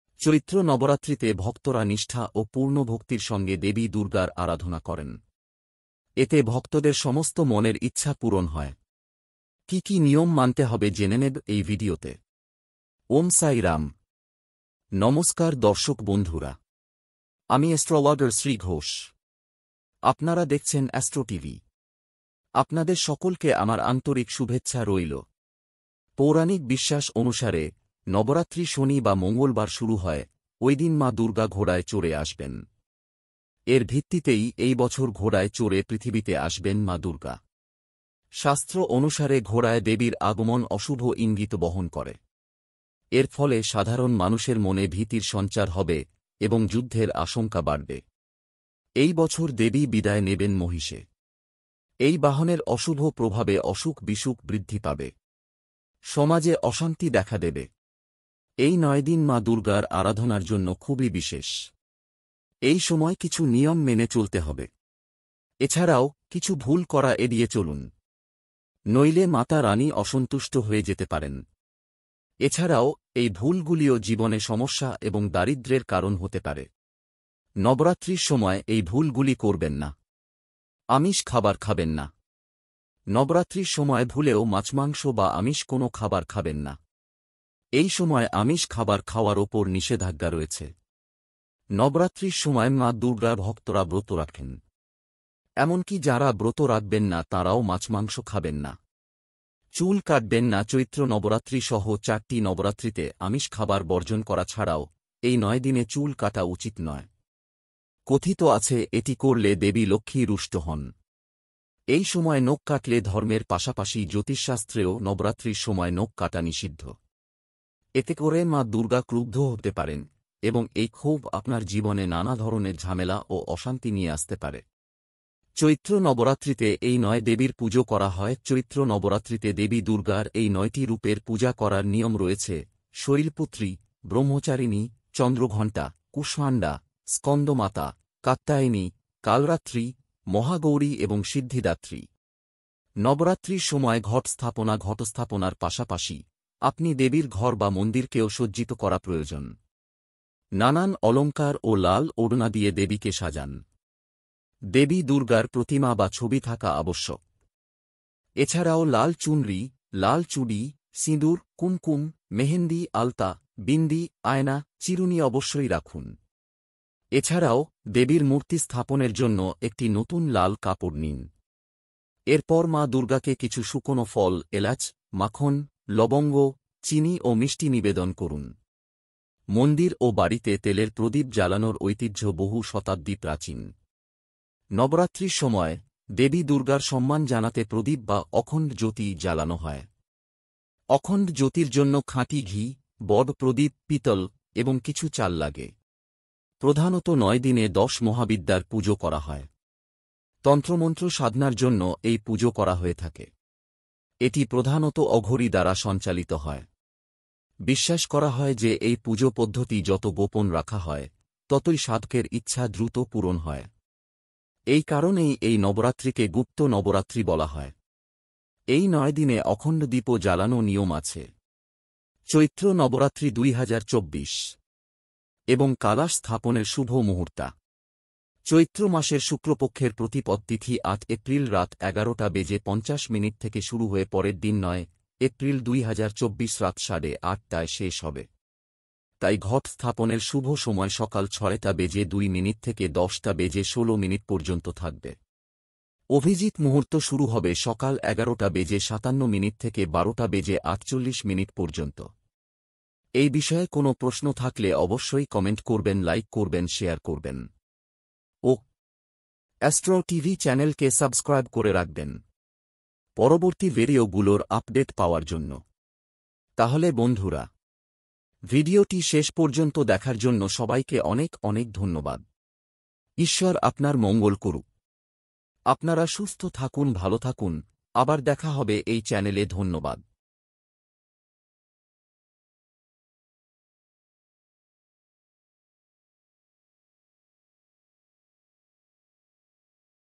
chaitra Navratri 2024:চৈত্র নবরাত্রিতে এই ভুলগুলিও জীবনে সমস্যা এবং দারিদ্র্যের কারণ হতে পারে।bangla
chaitra Navratri 2024
Navratri
Navratri 2024

দেবী ভাগবত্ পুরাণ অনুসারে গুপ্ত নবরাত্রি মা দুর্গার আরাধনা করলে জীবনের সব সংকট দূর হয়। গুপ্ত নবরাত্রিতে দেবী দুর্গার গোপনে আরাধনা করা হয়। দেবীর আসা ও যাওয়ার বাহন স্থির হয় সপ্তাহের কোন দিনে তা পড়ছে, তার উপর নির্ভর করে। এই বছর মাঘ গুপ্ত নবরাত্রিতে দেবী দুর্গার আগমন হবে ঘোড়ায়। শাস্ত্র অনুসারে ঘোড়ায় দেবীর আগমন অশুভ ইঙ্গিত বহন করে। এর ফলে সাধারণ মানুষের মনে ভীতির সঞ্চার হবে এবং যুদ্ধের আশঙ্কা বাড়বে। এই বছর দেবী বিদায় নেবেন মহিষে। এই বাহনের অশুভ প্রভাবে অসুখ বিসুখ বৃদ্ধি পাবে। সমাজে অশান্তি দেখা দেবে।